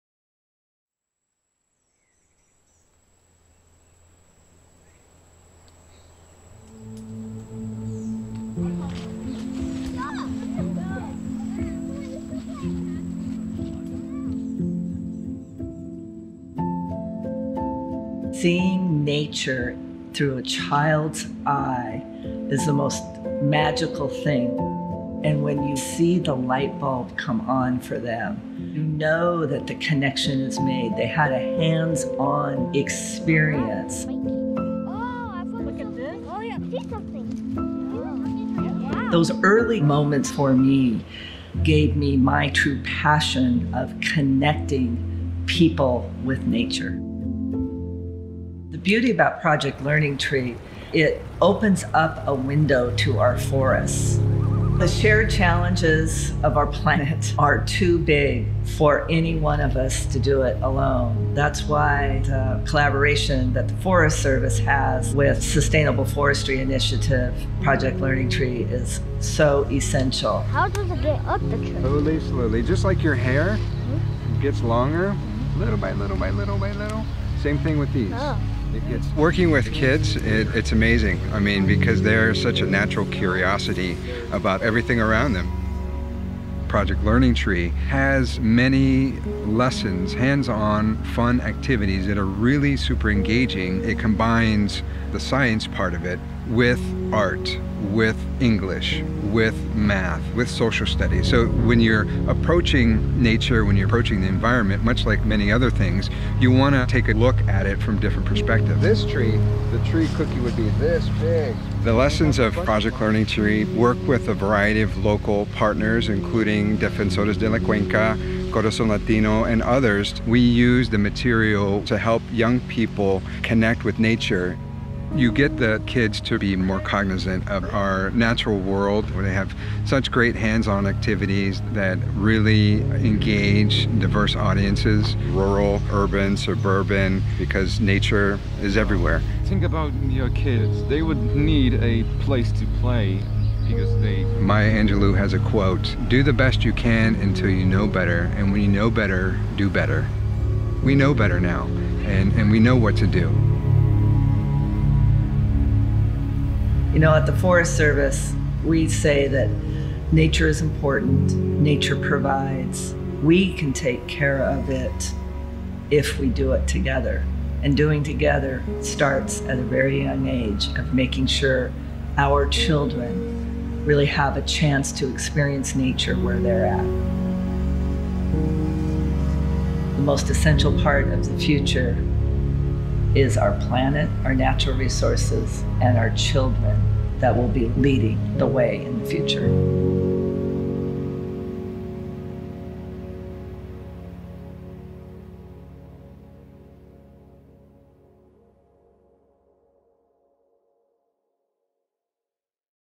Project Learning Tree's Forest Classrooms Helping Raise Responsible Children - Full Version
Project Learning Tree is proud to share a new video, produced in collaboration with BBC StoryWorks, and featuring USDA Forest Service Chief Vicki Christiansen and PLT-trained educators.

Initially presented by The Consumer Goods Forum as part of the "Better Lives Through Better Business" video series, we're thrilled to join the other organizations involved that are working to improve global sustainability and support future generations.